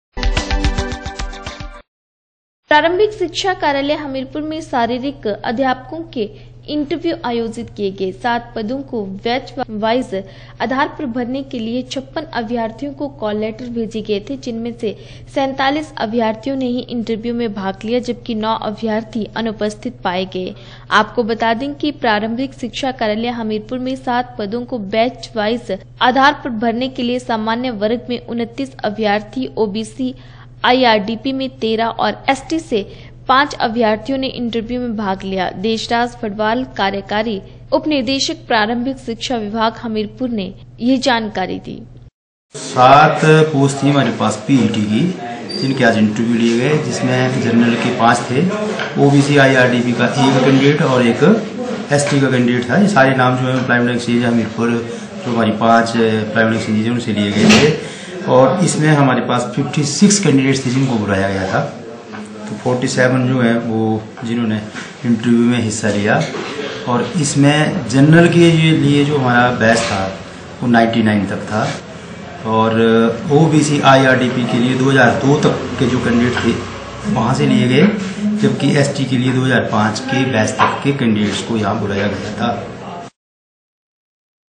हमीरपुर प्रारिम्भक शिक्षा कार्यालय हमीरपुर में शारीरिक अध्यापको के साक्षात्कार आयोजित
हमीरपुर प्रारिम्भक शिक्षा कार्यालय हमीरपुर में शारीरिक अध्यापको के साक्षात्कार आयोजित किए गए । सात पदो को बैंचवाईज आधार पर भरने  के लिए 56 अभ्यार्थियो को काॅल लेटर भेजे गए थे। जिसमें 47 अभ्यार्थियो ने साक्षात्कार में भाग लिया ।